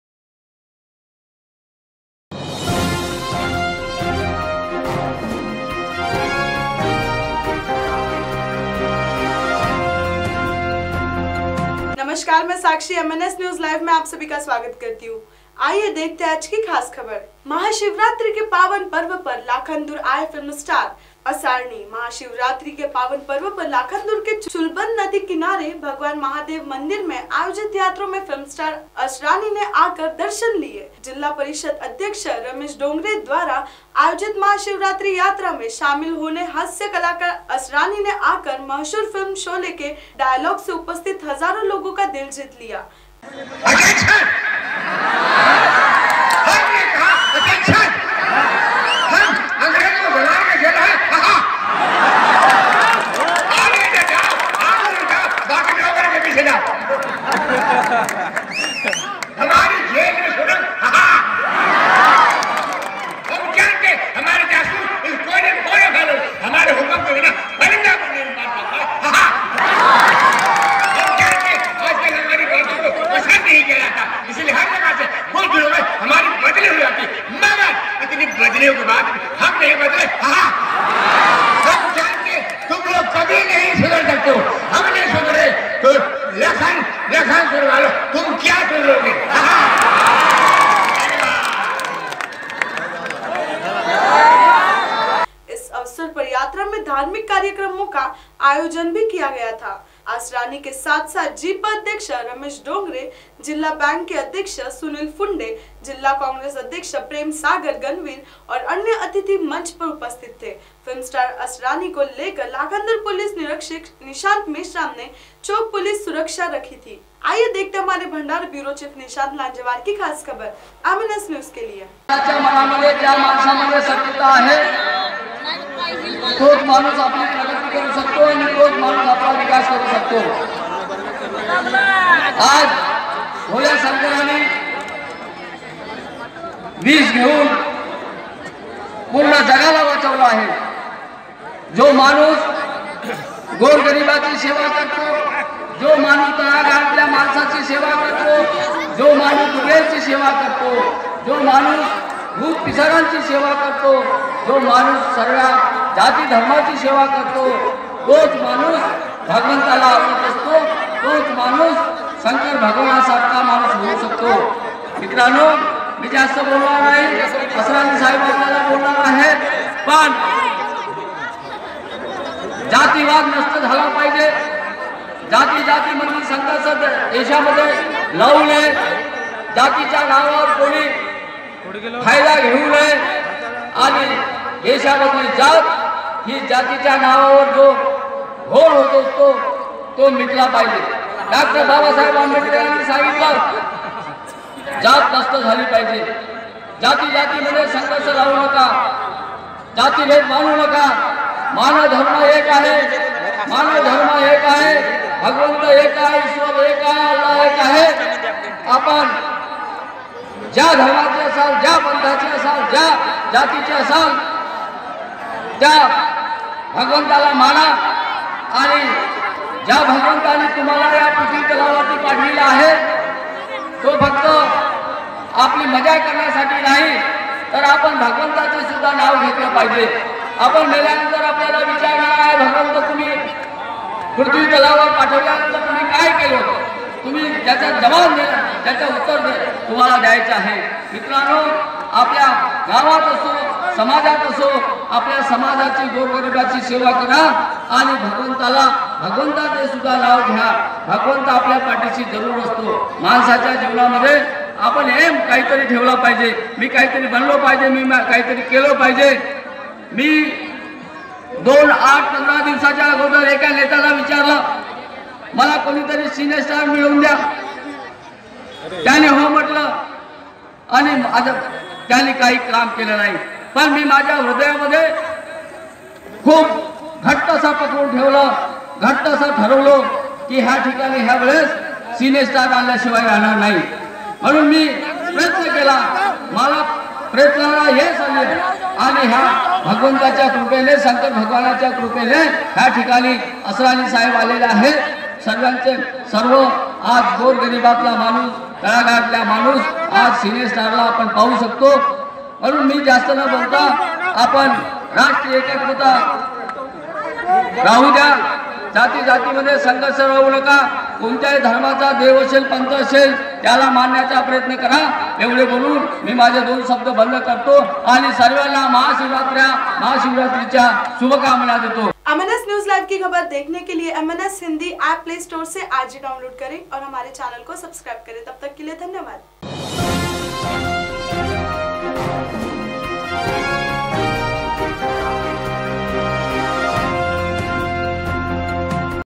MNS NEWS LIVE IN HINDI महाशिवरात्रि को लाखन्दुर में दर्शन लेने आये असरानी।
MNSNEWSHINDILIVE महाशिवरात्रि को लाखन्दुर में दर्शन लेने आये असरानी।